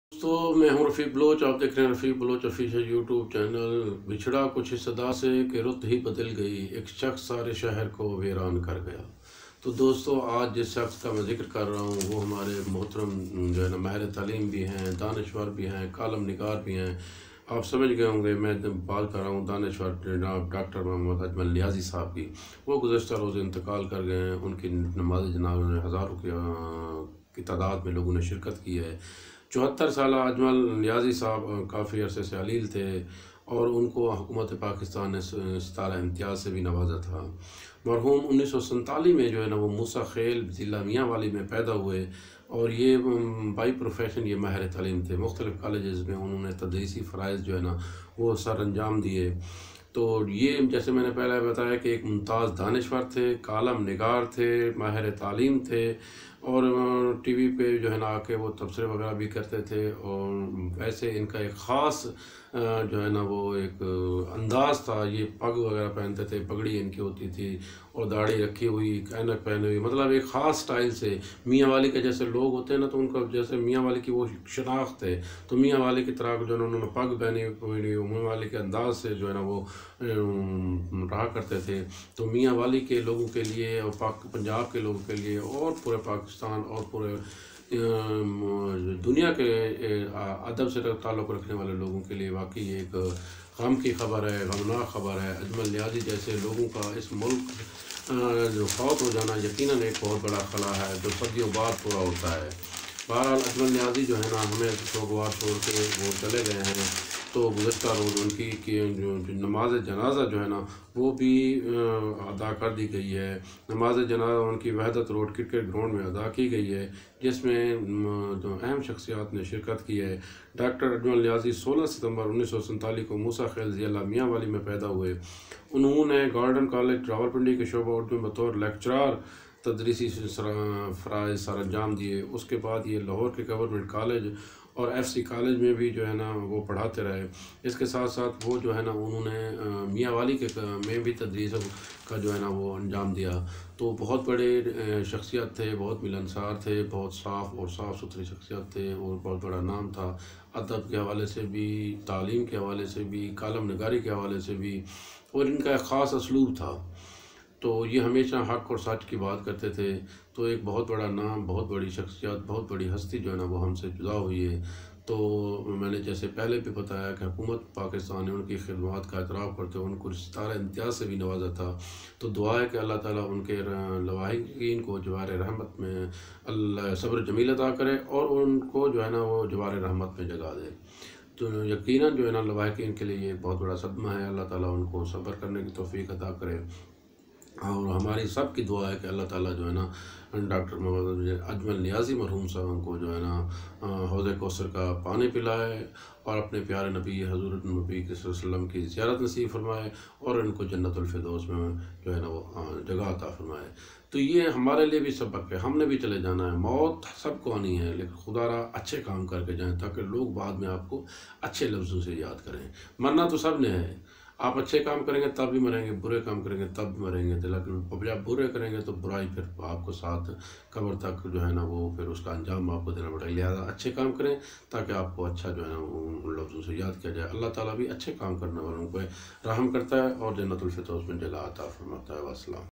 خبر غم ۔۔ڈاکٹر اجمل نیازی بھی اللہُ تعالیٰ کو پیارے ہوگئے۔ محمد رفیق بلوچ
In this video Rafiqbalouchofficial youtube channel speaker told about the sorrow news about Dr.ajmal niazi Shb. Rafiq Balouch official#dr.ajmal niazi death#Breaking news Pakistan